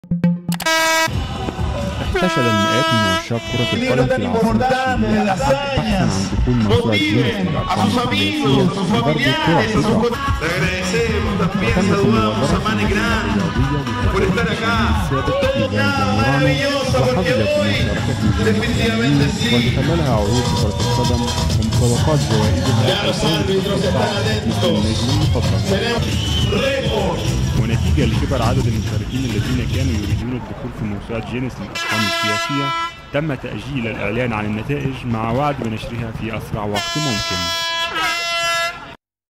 مسابقة لدخول موسوعة جينيس في ضرب الكرة بالرأس تجذب المئات في تشيلي
احتشد المئات من عشاق كرة القدم في العاصمة التشيلية الأحد بحثا عن دخول موسوعة جينيس للأرقام القياسية لضرب الكرة بالرأس.

وتمت المبادرة في المدينة الرياضية للهداف التشيلي السابق ايفان زامورانو وحظيت بمشاركة نجوم محليين، وتخللها عروض بكرة القدم ومسابقات لها جوائز الفوز بقصة شهر مثل النجم المفضل.